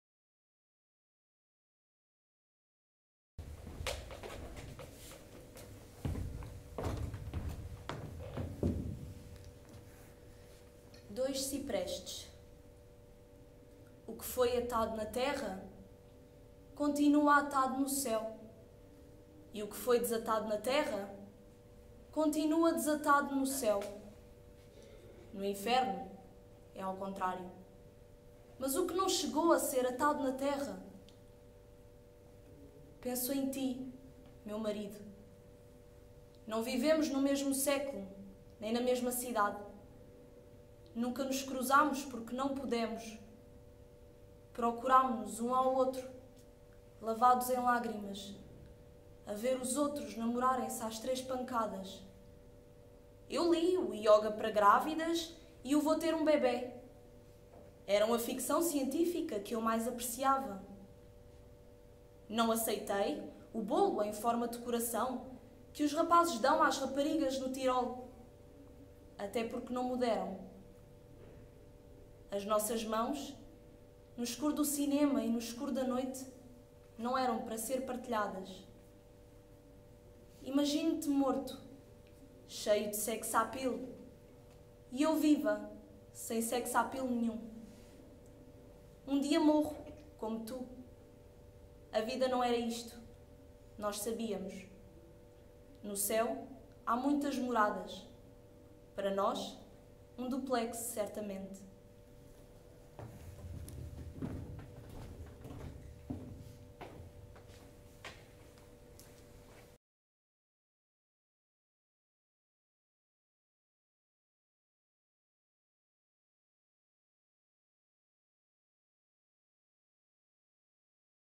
"Dois Ciprestes" Por Jessica Pires
Teatro da Palavra - Turma de interpretação 1º ano
“Dois Ciprestes” de ADILIA LOPES
Docente: Diogo Dória